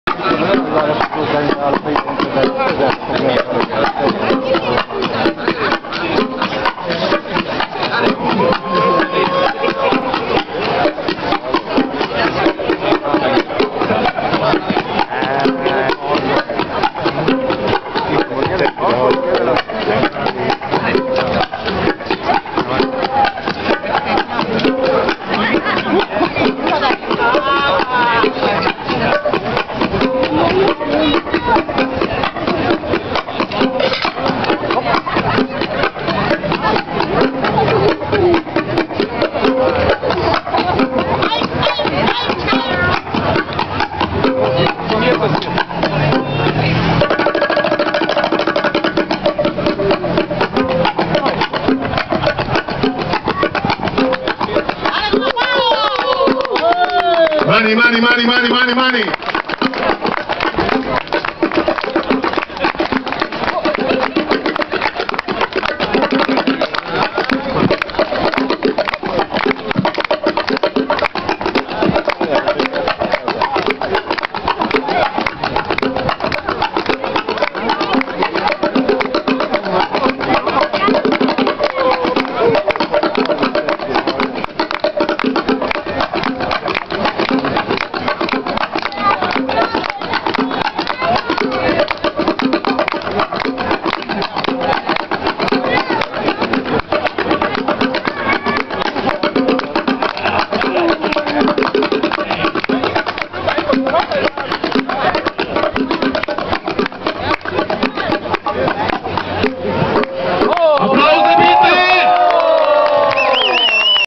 Fiesta de Cuba Passo Pramollo
30. Marzo 2008 en Nassfeld - Passo Pramolla era una fiesta muy divertida.